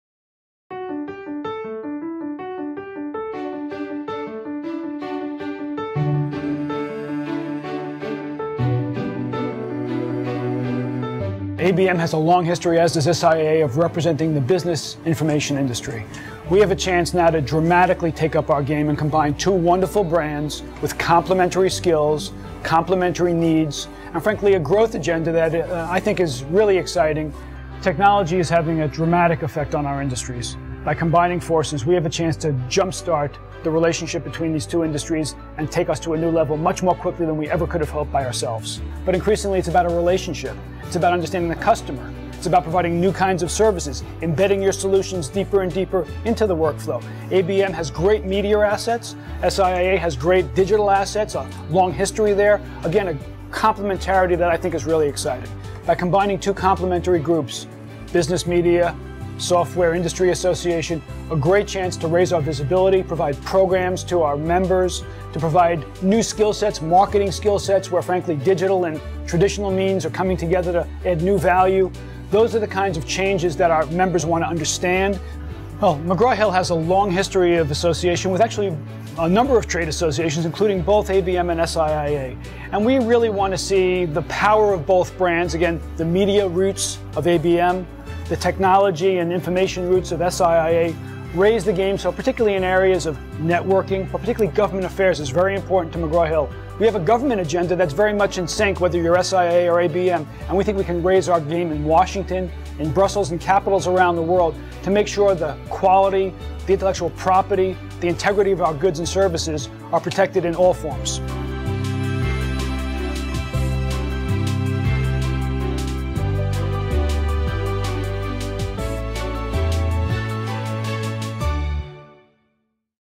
President of Commodities and Commercial Markets at The McGraw-Hill Companies on ABM + SIIA
"ABM and SIIA are in sync on regulatory agenda, so we can raise our game in Washington, in Brussels and capitals around the world to make sure our intellectual property is protected in all forms." -- Glenn Goldberg, President of Commodities and Commercial Markets at The McGraw Hill Companies, on ABM + SIIA